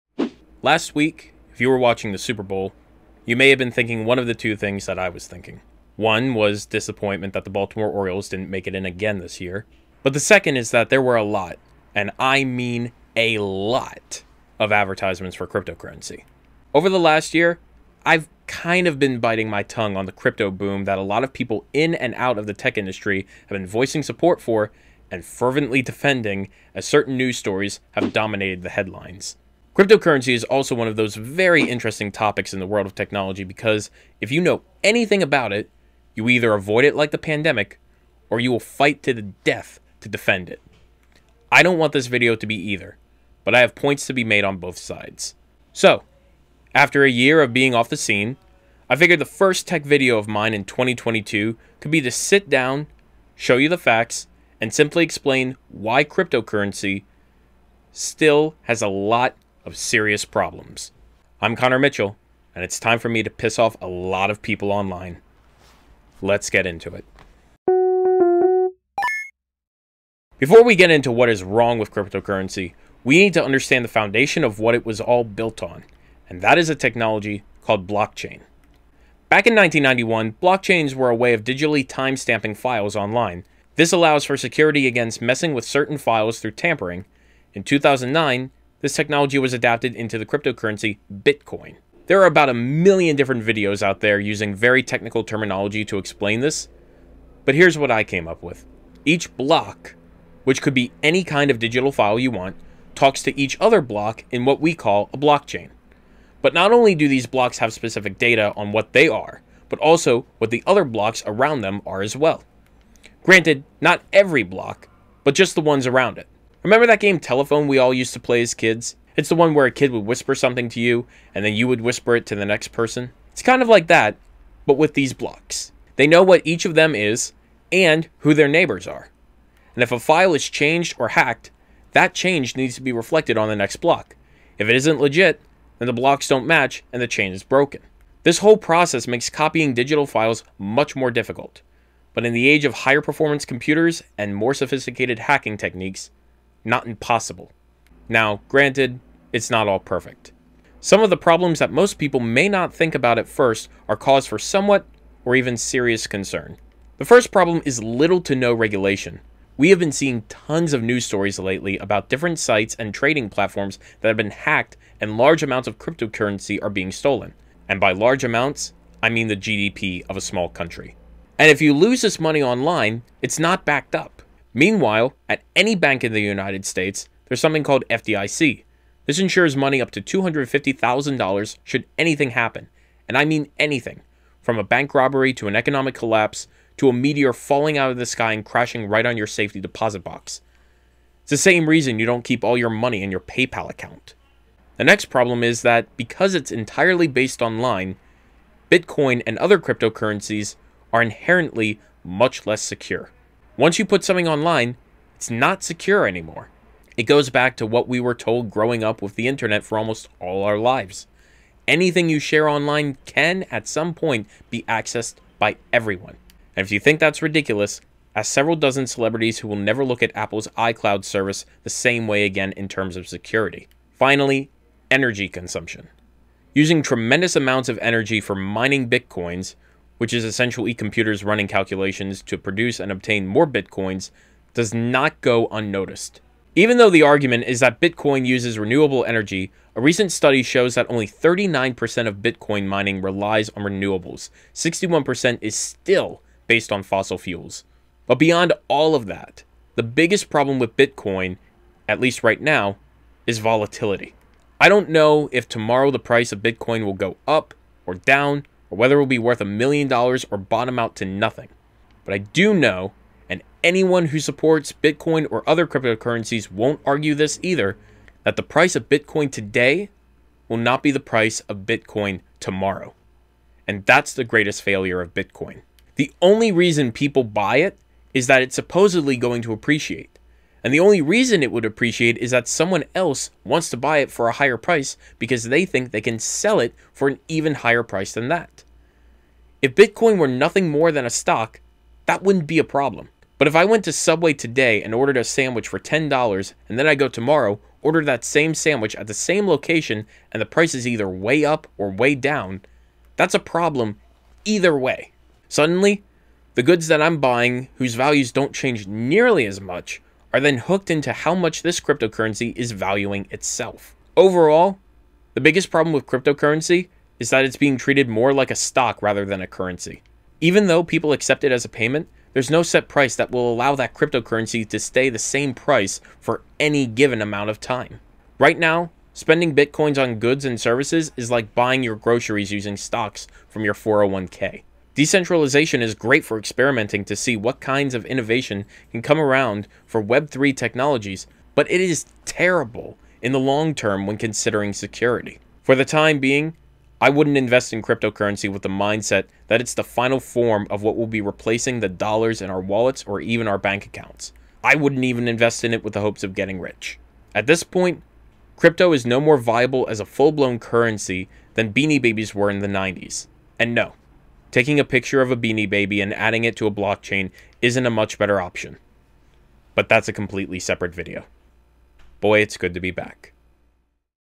The Problems With Cryptocurrency/Blockchains: A Simplified Explanation
With all the talk of BitCoin, NFTs, and other forms of Cryptocurrency, I decided it was finally time for me to give a simplified explanation on why I think there are still some core problems with this new form of currency.